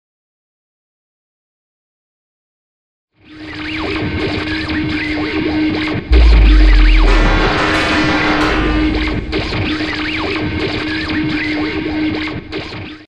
ARDE ROCK abertura antiga / abertura nova .
Abertura do canal da Arde Rock no YouTube, criada por Addler Martin no Estúdio Master. 24/06/2016.